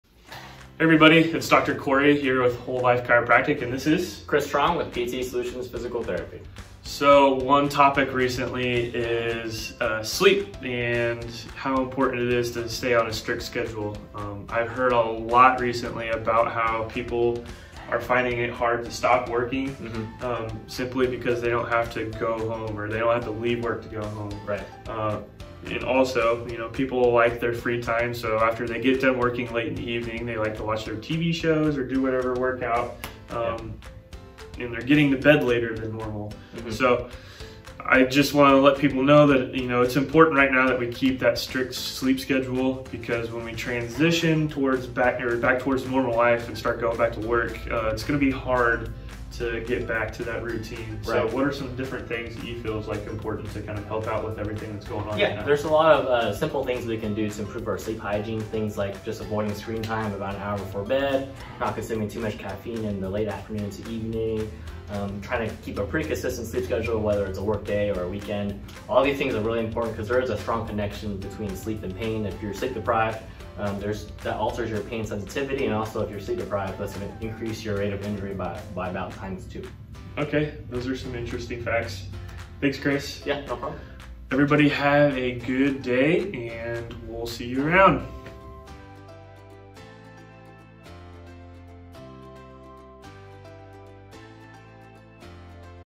WLC and PT Solutions Discuss the Importance of Sleep
Dr. Corey and Chris Truong, from PT Solutions in Leawood, discuss the importance of sleep. While many people have started back at their workplaces, there are still large numbers of people in the Kansas City area that are working from home. It is really important to stick to a schedule, especially when it relates to sleep.